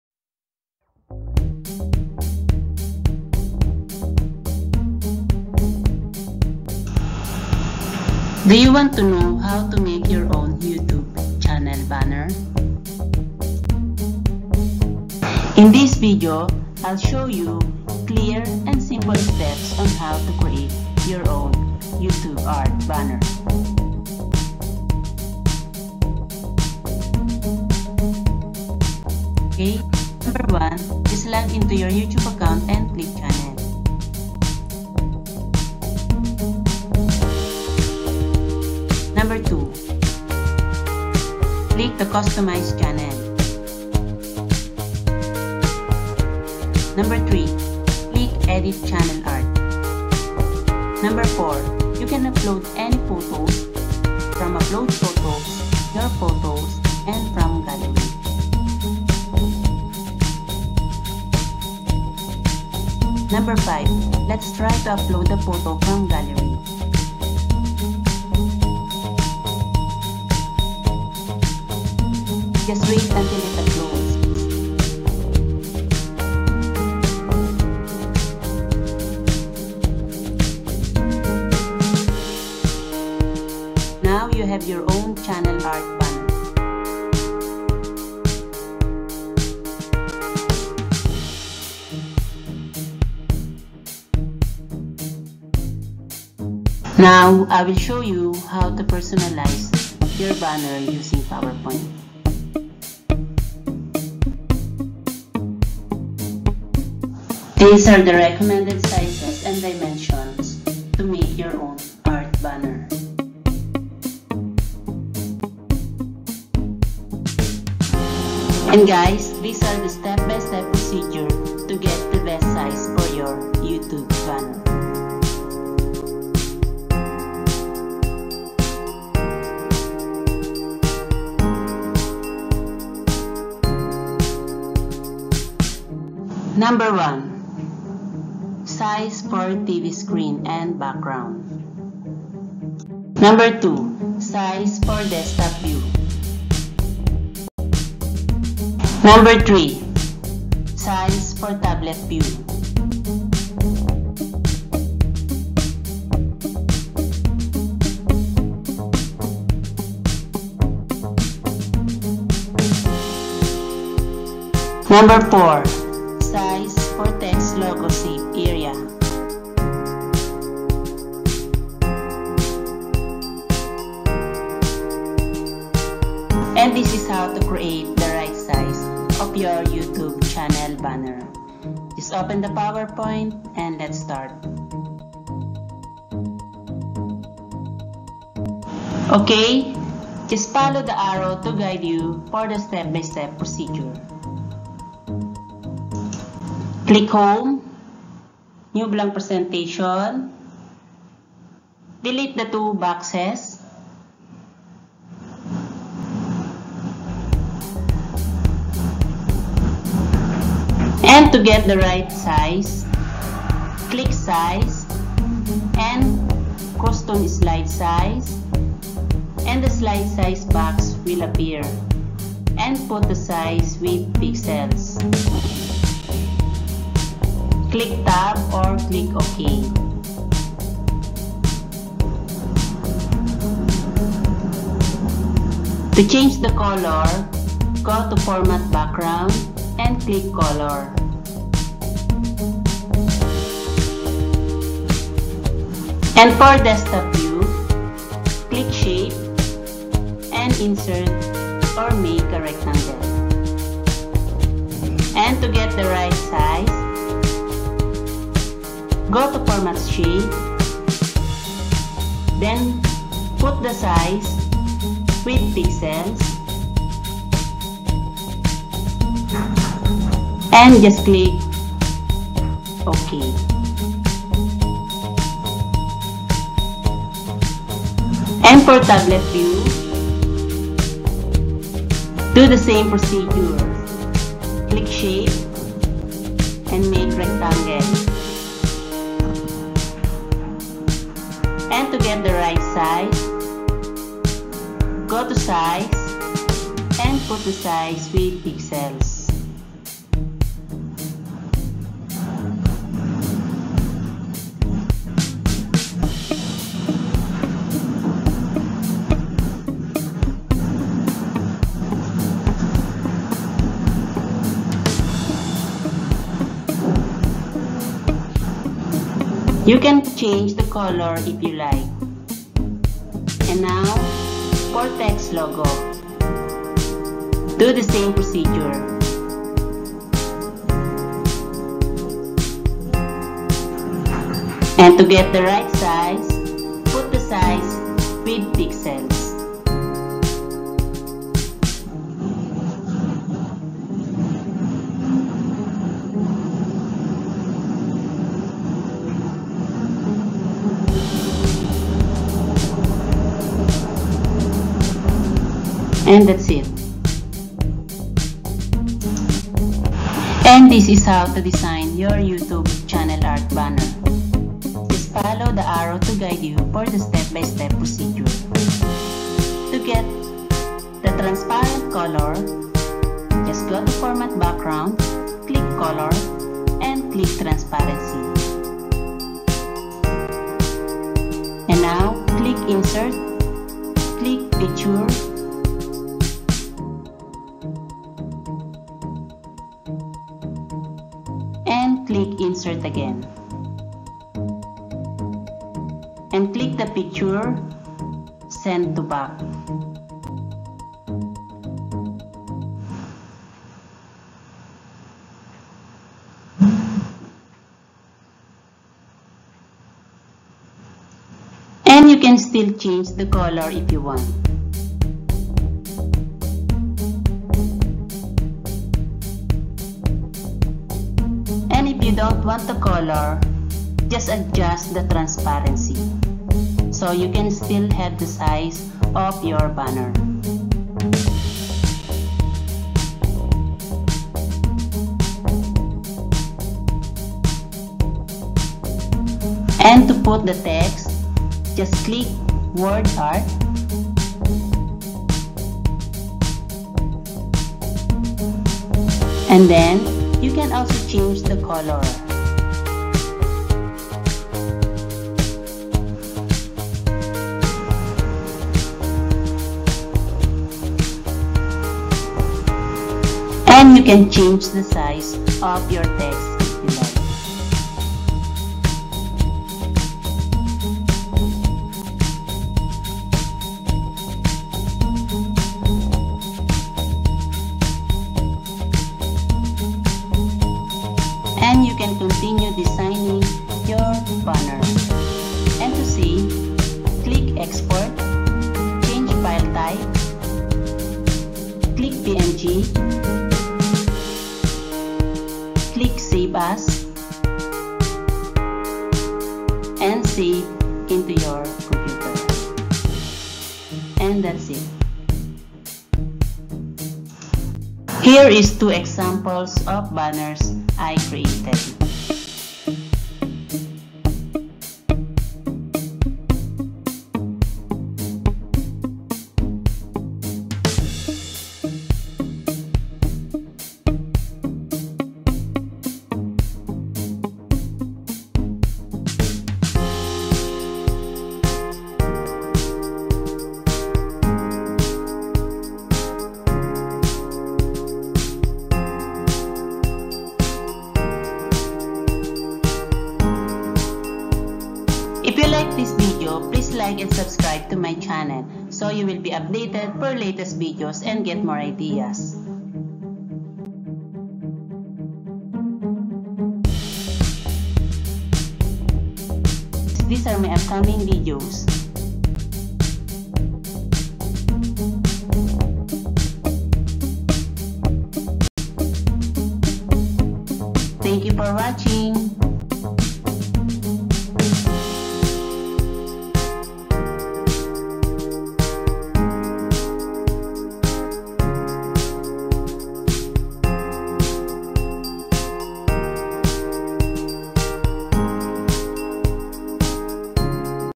How to make a Youtube Art Channel Banner using Powerpoint
VLOG TITLE: How to make a youtube channel art banner using powerpoint

This video is about channel banner tutorial. It shows the channel art size and explains in ENGLISH and TAGALOG

Pls check all my video it might be helpful for you!

Im very happy and appreciate your visit !Thank you so much!

ma sel technet & digital arts